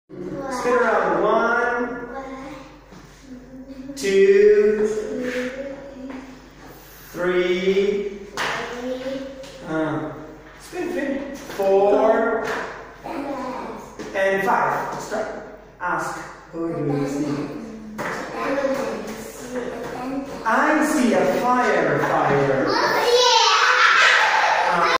24- Blindfold Game | Muxi's ESL classroom fun game | Teaching in China
Fun flashcard English learning game.
Have the students sitting in a circle, having flashcards. one student blindfolded in the middle of the circle , spin around for 5 times and whoever he is facing , ask a question , that student has to answer or use that flashcard in a sentence. now it's that students turn.

Like my Facebook page:
     "Muxi's ESL World"

      " English Teaching Games by Muxi "

Enjoy the video and write your opinion in the comments section below